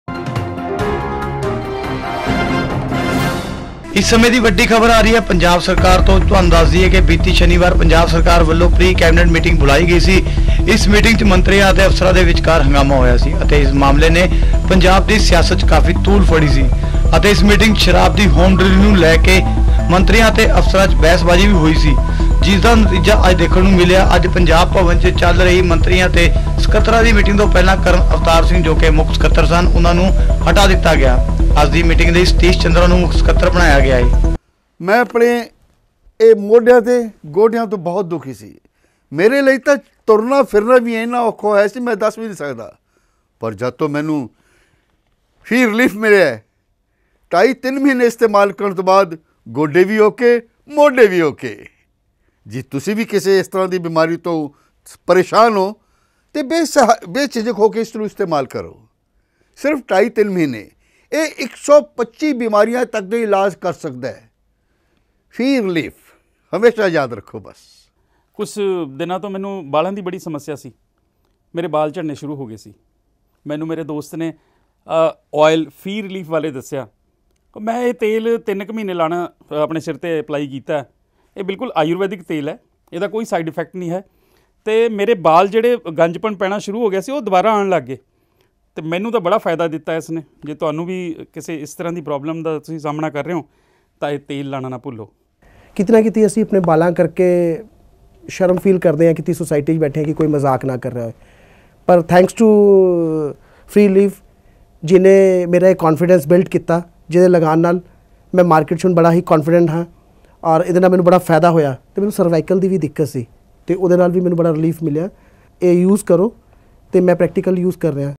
Breaking News | ਪੰਜਾਬ ਸਰਕਾਰ ‘ਚ ਕਰਨ ਅਵਤਾਰ ਨਹੀਂ ਰਹੇ ਮੁੱਖ ਸਕੱਤਰ | AOne Punjabi Tv
ਮੀਟਿੰਗ ਤੋਂ ਪਹਿਲਾਂ ਲਗਾਇਆ ਸਤੀਸ਼ ਚੰਦਰਾ ਨੂੰ ਮੁੱਖ ਸਕੱਤਰ
ਪੰਜਾਬ ਸਰਕਾਰ ਨੇ ਮਾਮਲਾ ਵੱਧਣ ਤੋਂ ਬਾਅਦ ਦਿਖਾਈ ਸਖਤਾਈ |